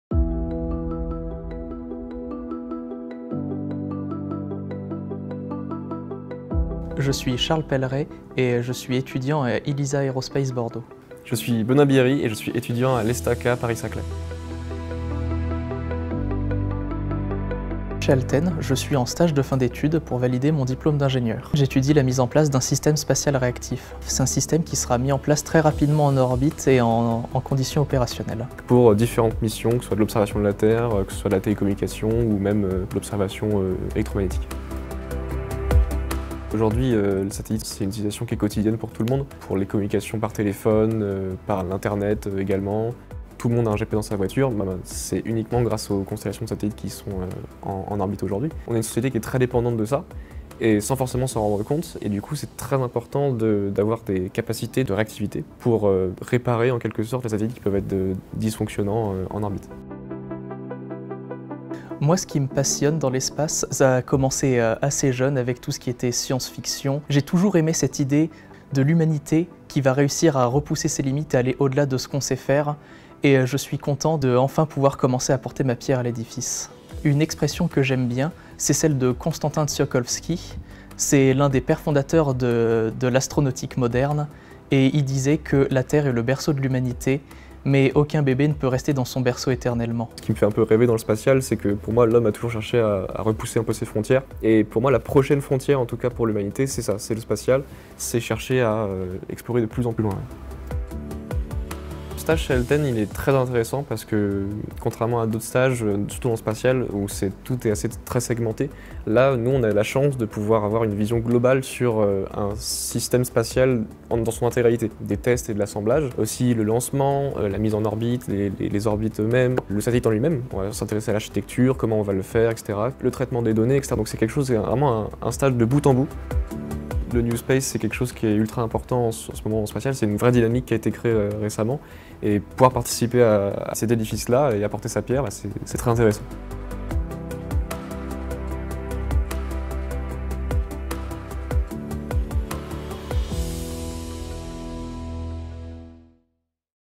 Space Innovation Lab: meet Benoît & Charles
Rencontre avec Benoît et Charles, étudiants stagiaires chez ALTEN. Ils nous partagent leur vision sur l'avenir du secteur spatial 🎤